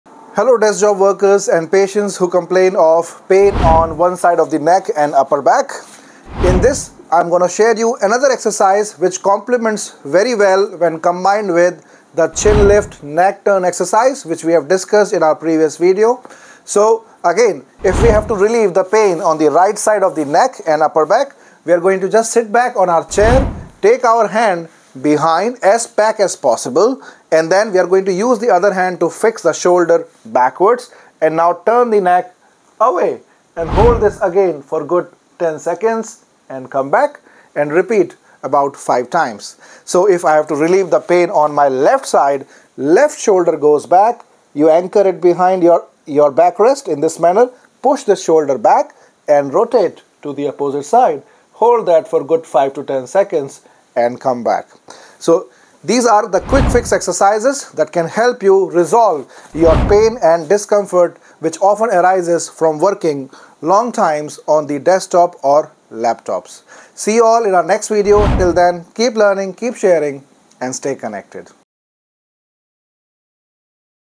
DIY -Quick Fix for Neck and Upper Back Pain: Get Instant Comfort.
For all Desk Job workers, This simple exercise will help stretch and activate your inhibited muscles around the head and neck region giving you an instant relief and enhanced support while working on computers for long.
Do let us know how it has helped you in comments !! ALL THE BEST !!